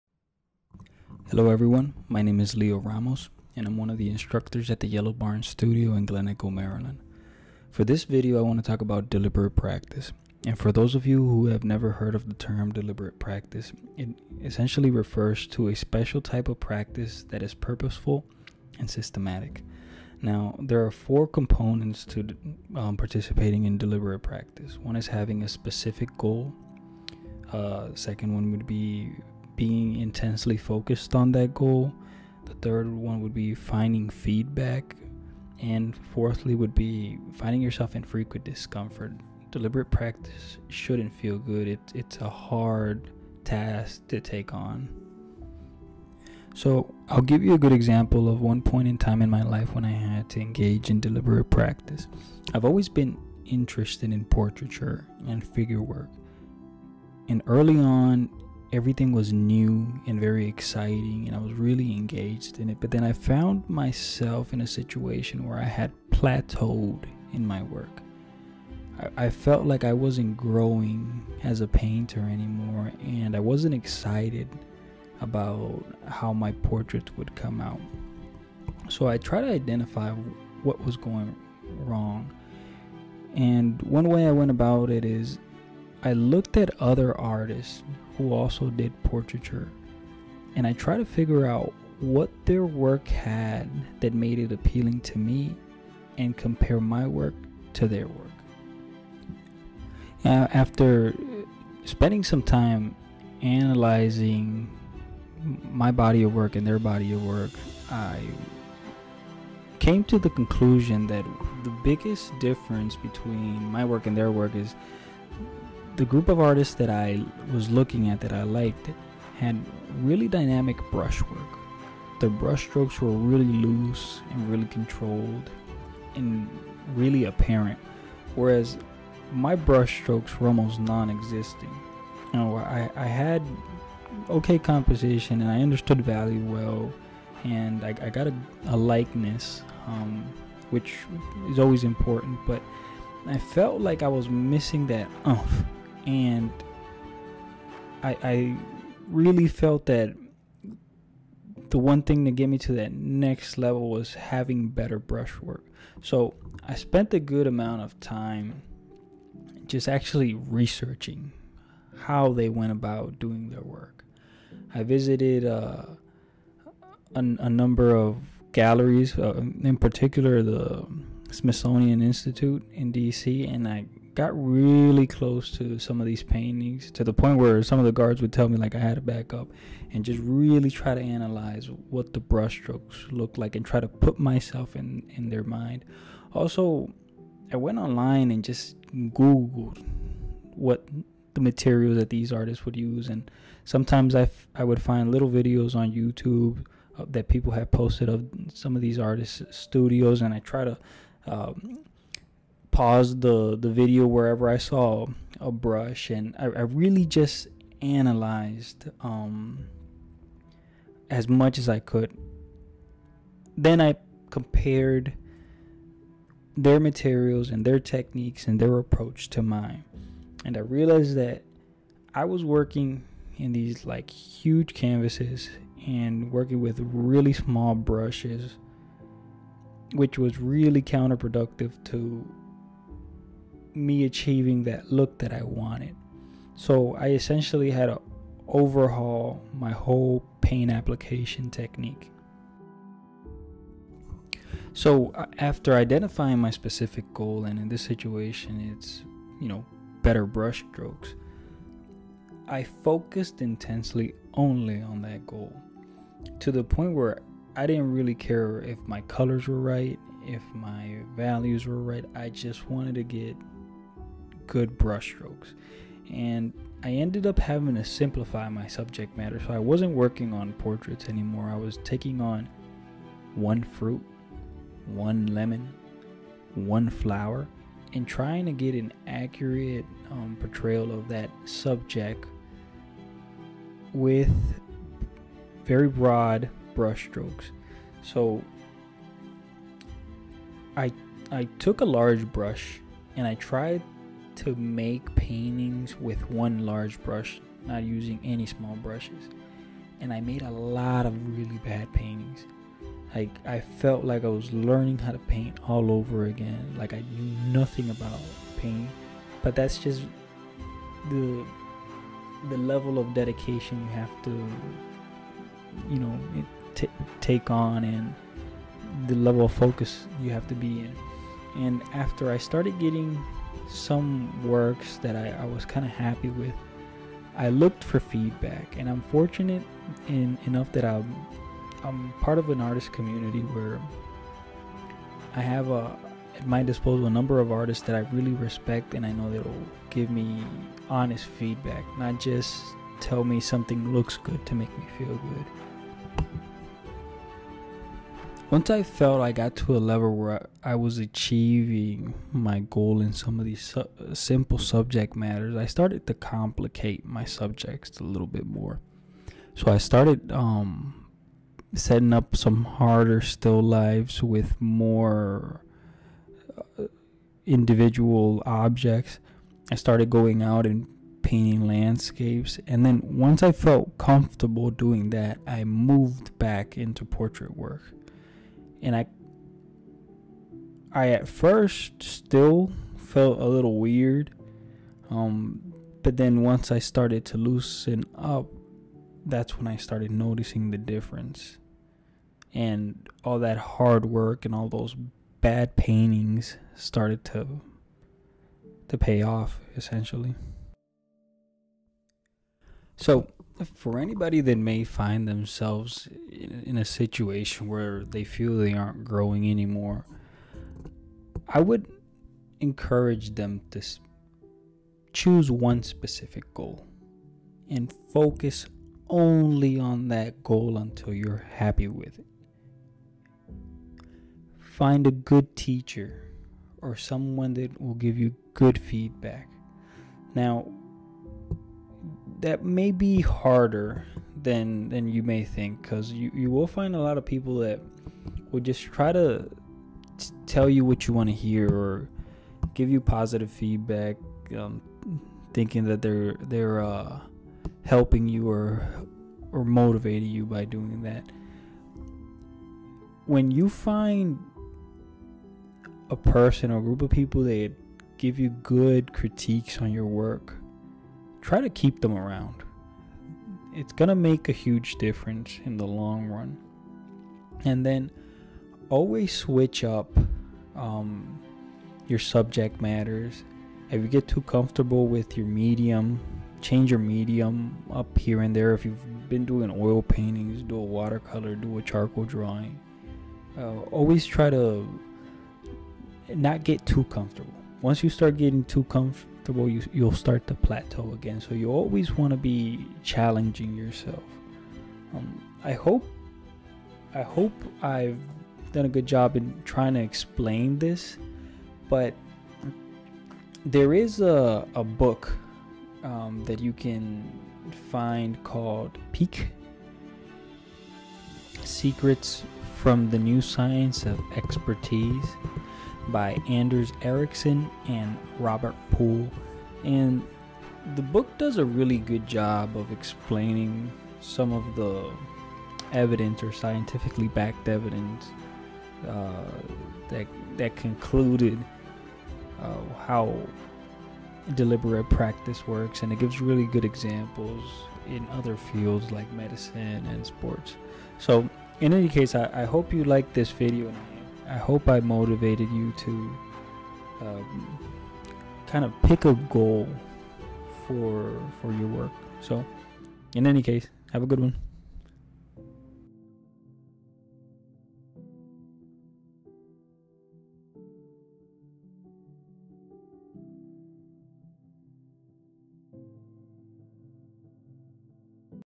Applying deliberate practice to your art work
In this video I talk about applying deliberate practices to grow as an artist.